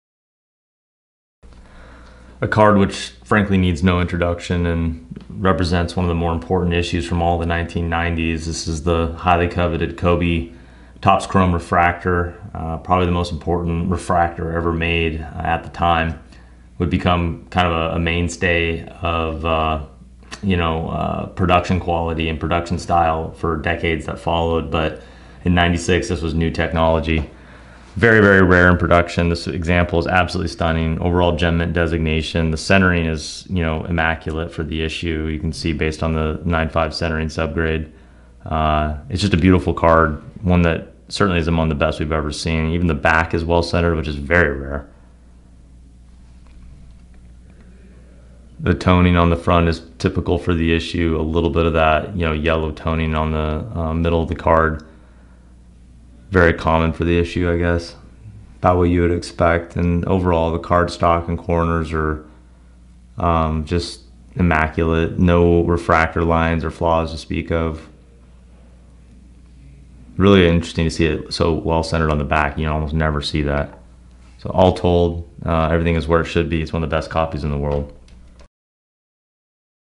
1996 Topps Chrome Refractor Kobe Bryant ROOKIE #138 BGS 9.5 GEM MINT
Safely secured in a BGS holder is one of the most important rookie cards from the decade--Kobe Bryant's 1996 Topps Chrome Refractor. Even the most novice of collectors is aware of this card's condition issues, often plagued by heavy greening, surface wear, and centering shifts. As a result, this card seldom earns Beckett's overall '9.5' grade. It did so here and is currently one of only 88 examples in existence. Supply cannot meet demand as millions desire to own this card. The offered specimen boasts spectacular centering, pin-point sharp corners, and edges which lack chipping. A lone surface indent as well as typical greening accounts for the lone '9' sub. As the 13th pick in the draft, we did not know at the time that Kobe would become one of the most electrifying athletes in sports history. Bryant dazzled us on the court for 20 seasons, memories of his play will continue to motivate us to reach our full potential, and this card will likely always be regarded as one of the best Kobe rookie cards to own. In August 2022, Netflix is releasing a 10-part documentary honoring Kobe’s legacy. We suspect this documentary will have the same effect on his trading cards as Michael Jordan's "The Last Dance" documentary. This card has high appreciation potential. PWCC recommends it without hesitation.